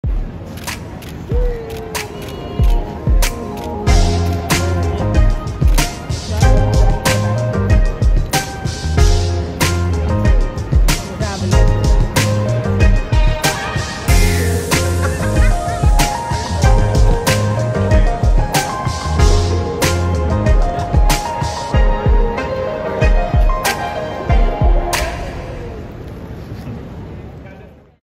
Posing for Her Life: Christine Quinn Selling Sunset Balenciaga Edition
Selling Sunset’s Christine Quinn was selling that garment like she made it herself. She was giving it her all and then some as photographers clicked away during arrivals at the Balenciaga Spring 23 Show at NYSE in NYC.